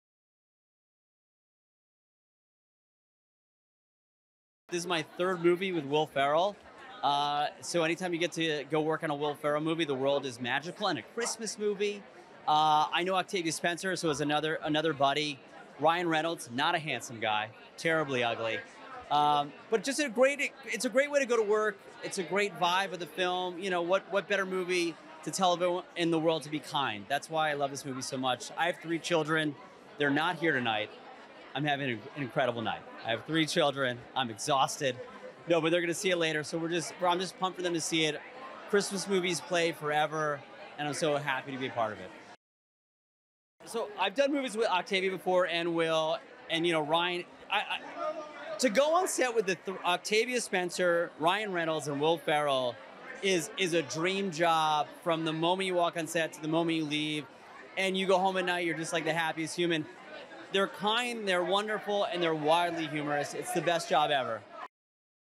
SPIRITED Interview by PJ Byrne
SPIRITED movie comedy pjbyrne SPIRITED Interview by PJ Byrne.
ABOUT:  Each Christmas Eve, th Ghost of Christmas Present (Will Ferrell) selects one dark soul to be reformed by a visit from three spirits. But this season, he picked the wrong Scrooge. Clint Briggs (Ryan Reynolds) turns the tables on his ghostly host until Present finds himself reexamining his own past, present and future. For the first time, “A Christmas Carol” is told from the perspective of the ghosts in this hilarious musical twist on the classic Dickens tale. From Director Sean Anders ("Daddy’s Home,” "Instant Family”), written by Sean Anders and John Morris, with original songs by Oscar winners Benj Pasek and Justin Paul ("La La Land") and production numbers choreographed by Chloe Arnold (“The Late Late Show with James Corden”), this modern retelling is a sweet and spicy holiday treat.

Starring:
Will Ferrell, Ryan Reynolds, Octavia Spencer, Sunita Mani
Directed By:
Sean Anders
Produced By:
Sean Anders, John Morris, George Dewey, Will Ferrell, Jessica Elbaum, David Koplan
Executive Produced By:
Diana Pokorny, Justin Paul, Benj Pasek